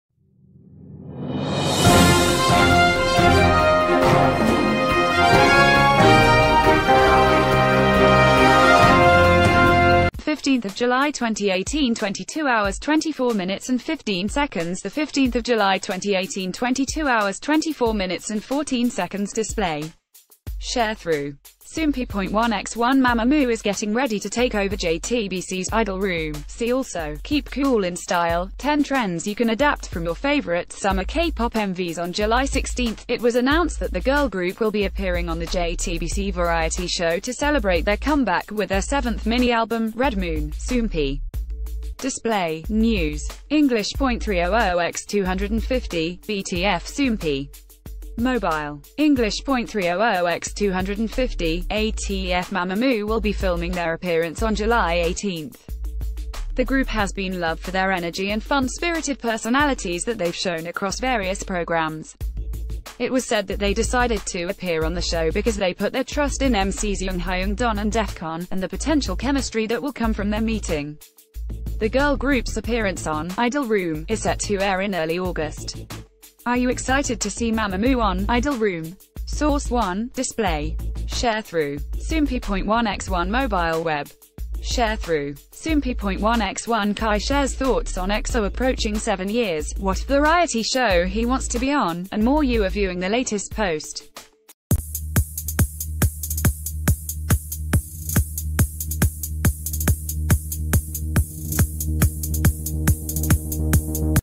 MAMAMOO Confirmed To Appear On JTBC's "Idol Room"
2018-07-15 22:24:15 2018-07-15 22:24:14 Display. Sharethrough. Soompi.1x1 MAMAMOO is getting ready t...